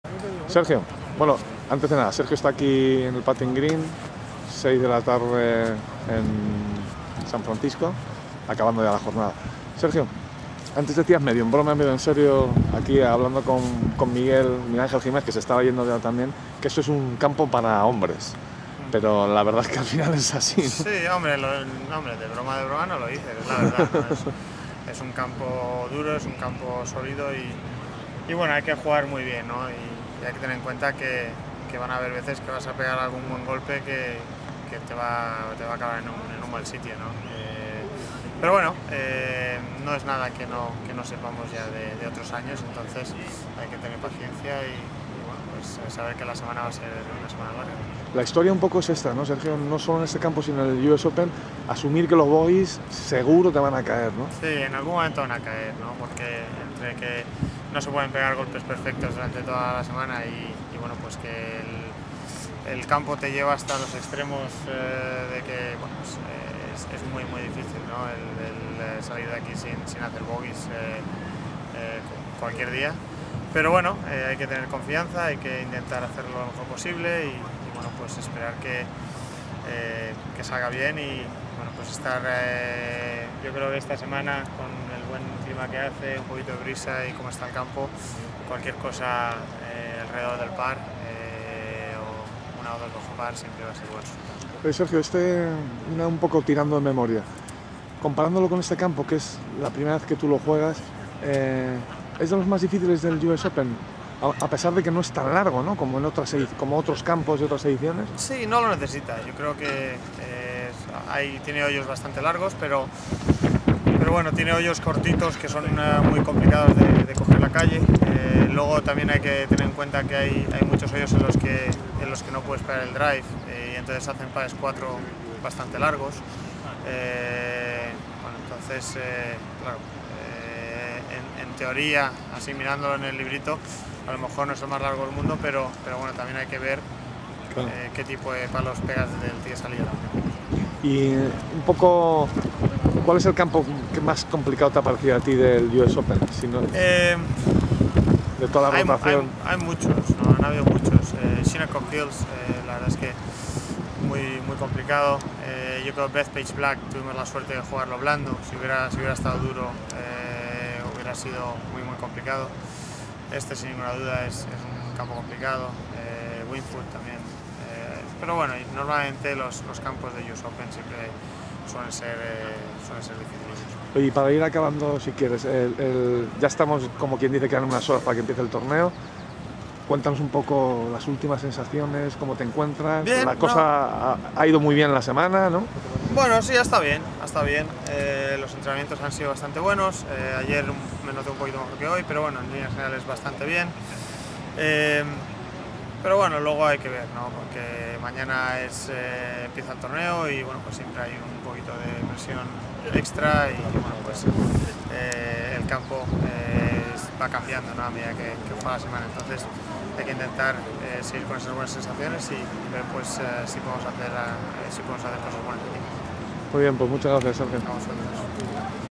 Los campos más difíciles del US Open, por Sergio García
Sergio García describe los campos más difíciles que ha jugado en el US Open. En la previa del US Open de 2012 en el Olympic Club, putting green.